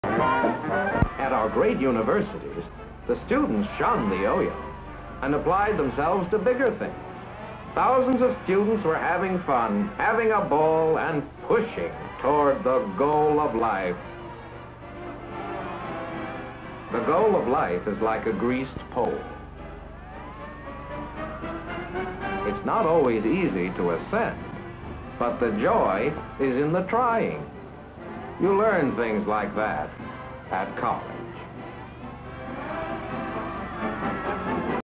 1960s Columbia University Initiations
Reel #: 1213

College boys playing with a giant rubber ball; others trying to climb a greased pole; they rip up their shirts while playing flag football.

Time: 1960s

Setting: Columbia University, New York City, New York

People: College Students, Columbia University Students

Objects: greased pole climbing, big rubber ball, flag football,

Subjects: hazing, Fraternity initiations,  1960s Columbia University,